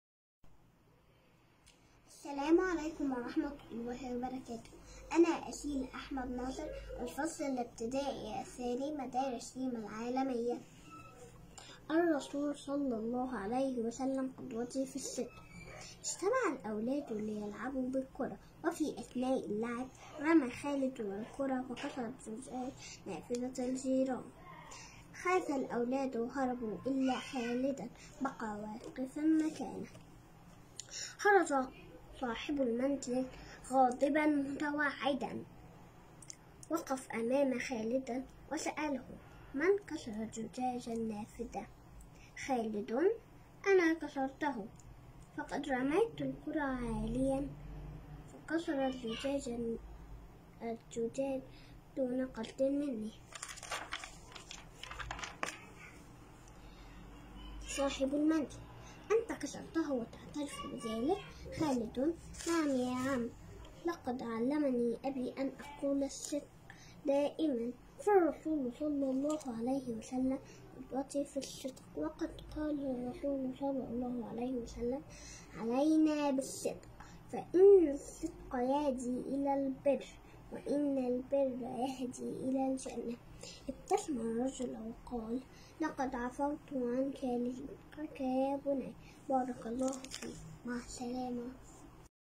Reema_International_School قراءة
طلاب الصف الأول والثاني والثالث والقراءة المعبرة